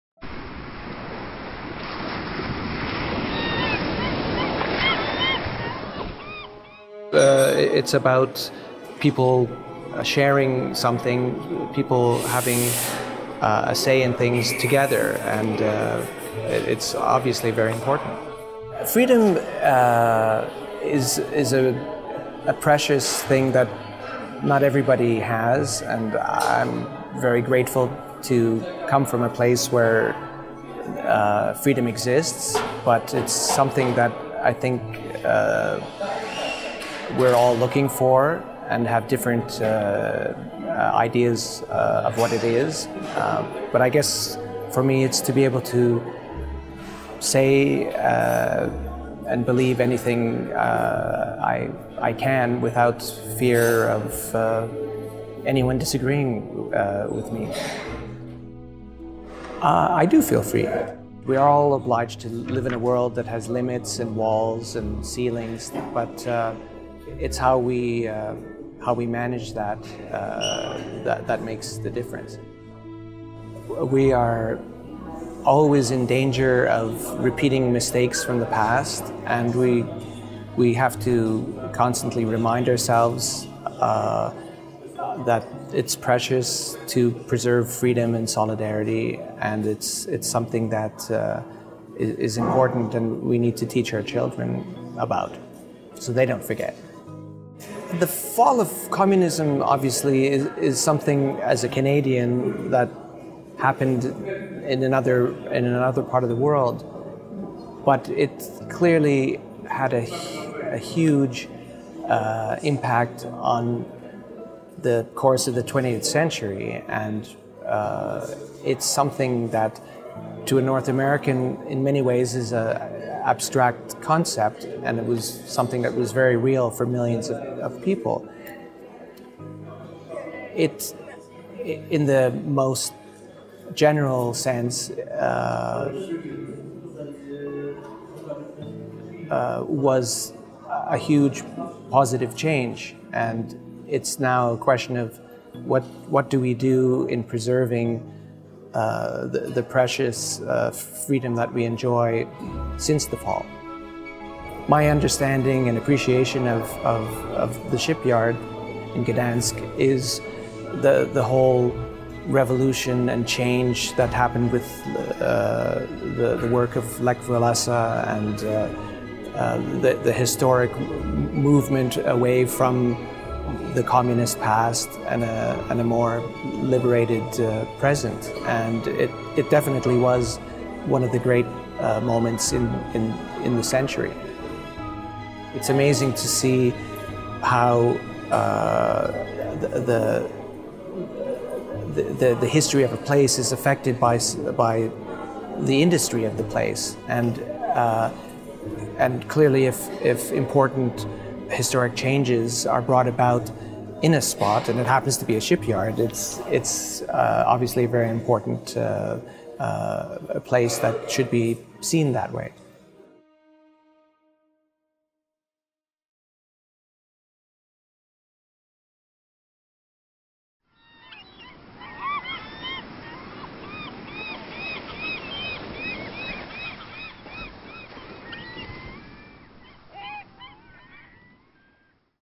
Gdańsk Shipyard// UNESCO// Paul Sarossy
Stocznia Gdańska w 2020 roku będzie polskim kandydatem do wpisania na prestiżową Listę światowego dziedzictwa UNESCO. Narodowy Instytut Dziedzictwa, koordynator projektu, zainicjował akcję, która ma zachęcić Polaków do kibicowania tej inicjatywie i przypominać, jak ogromną rolę odegrało to miejsce we współczesnej historii.

Do inicjatywy dołączyli słynni filmowcy, którzy byli gośćmi zeszłorocznego festiwalu Cameraimage w Toruniu . Wystąpili oni w serii wywiadów, w których mówią o wadze wolności i solidarności oraz przypominają o tym, że Stocznia Gdańska to kolebka tych wartości.